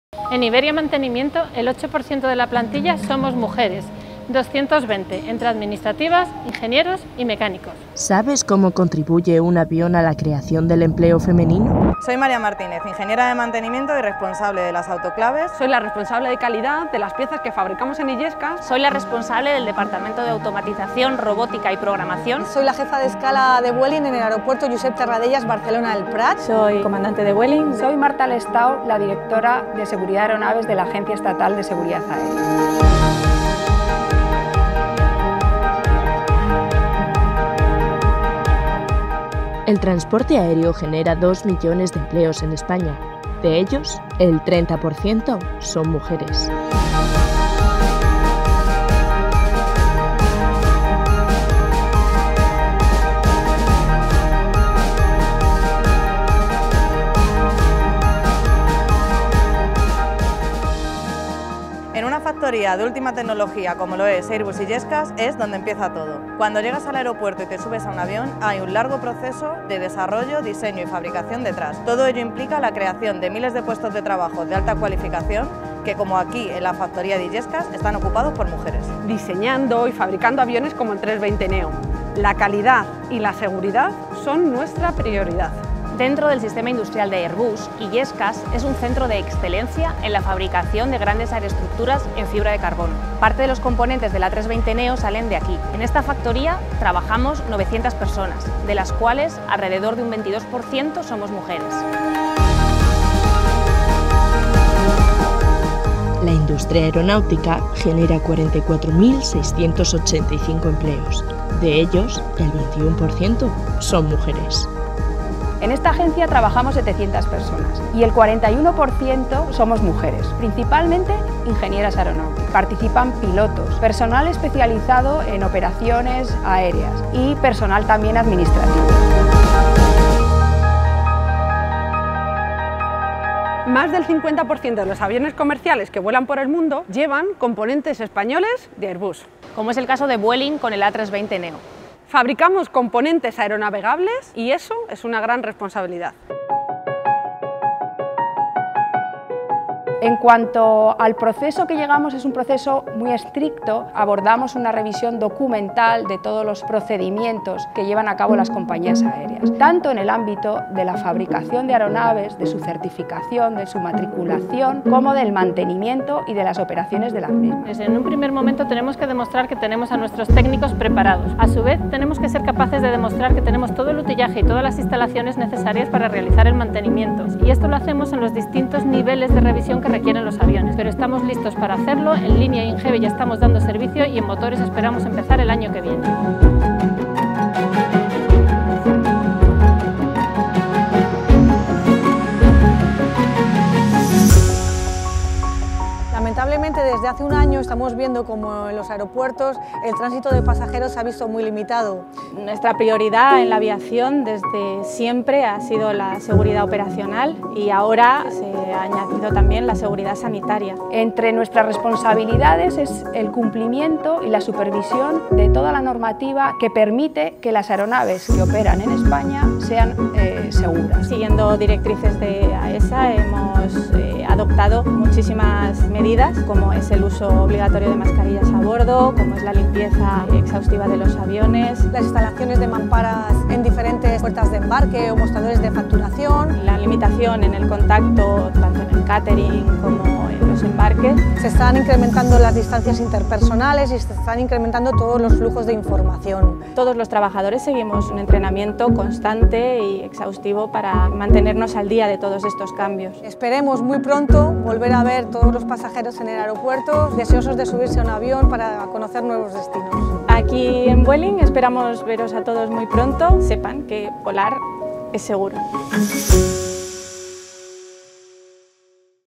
Festa Della Donna
Sai come un aereo contribuisce alla creazione di occupazione femminile? Scoprilo in questo video di dipendenti di Vueling, Iberia, Airbus y AESA! Un ringraziamento a Ellas Vuelan Alto per contribuire a dare visibilità al talento femminile e ispirare le future generazioni!